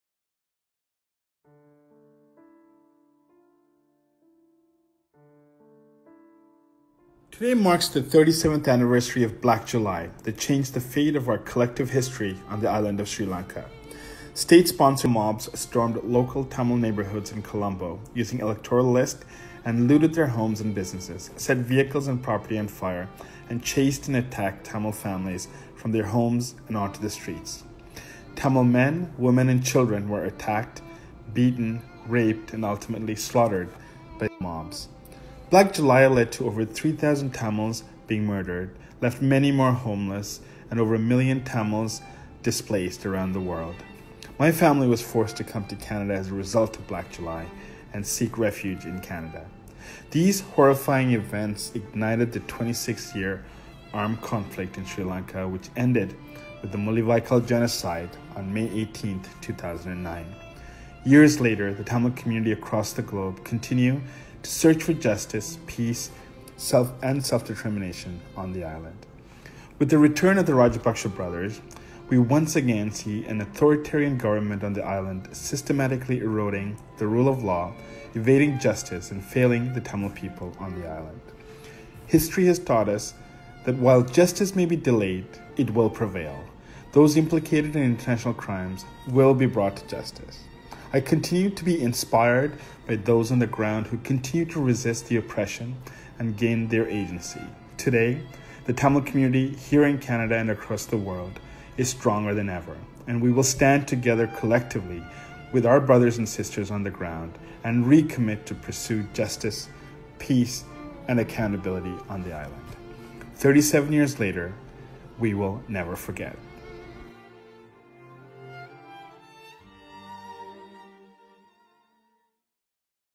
Black July கருப்பு ஜூலை 37வது ஆண்டு நிறைவையொட்டி: கரி ஆனந்தசங்கரி M.P Scarborough—Rouge Park
Today, we mark 37 years since the state-sponsored anti-Tamil pogrom of 1983 on the island of Sri Lanka.
Angry mobs, armed with electoral lists, brutally attacked Tamil civilians, plundered homes, set fire to houses, vehicles and people-alike while government authorities watched on. Over 3000 Tamils were dead over the span of a few days and over a million Tamils were displaced across the globe - including my family and I who sought refuge in Canada.
Black July became a catalyst to the 26-year armed-conflict in Sri Lanka which ended with the Mullivaikkal genocide of Tamils on May 18th 2009.
Thirty seven years on, we continue to seek justice and accountability. While justice may be delayed, it will prevail. Tamil